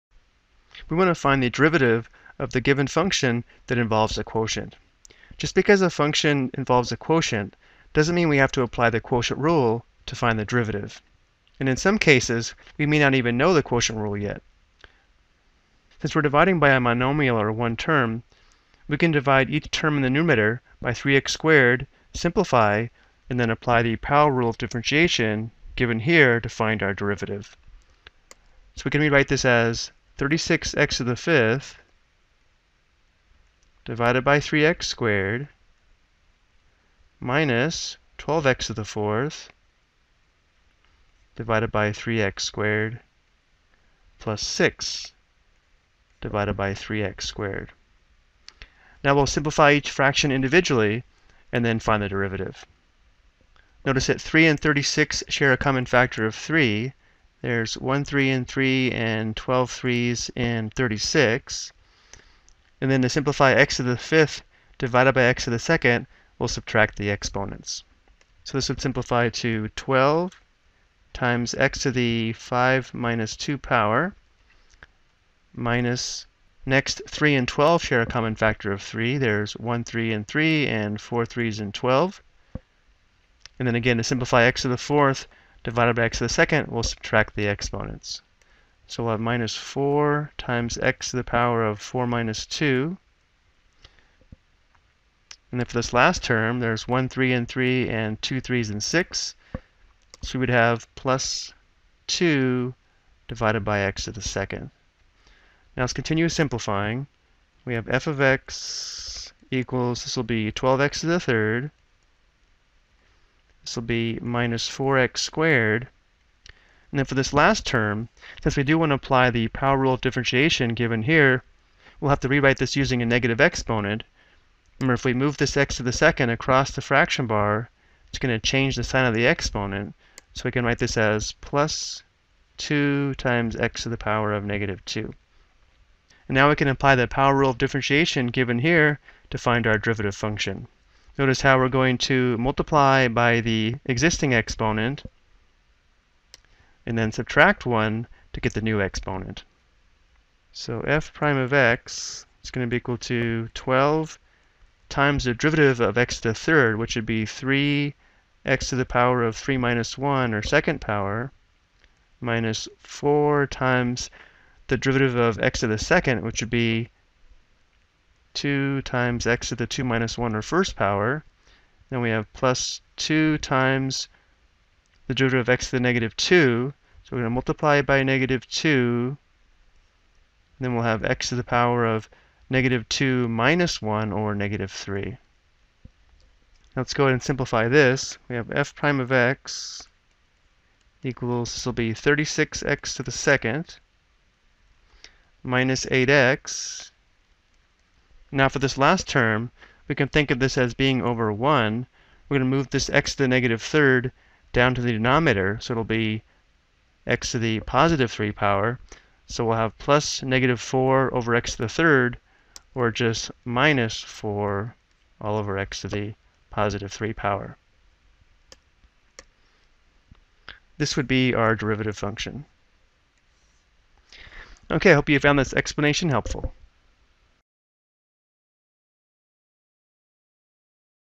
Ex: Derivative of a Quotient Function By Simplifying
This video explains how to find the derivative of a rational function by simplify, which requires dividing by a monomial.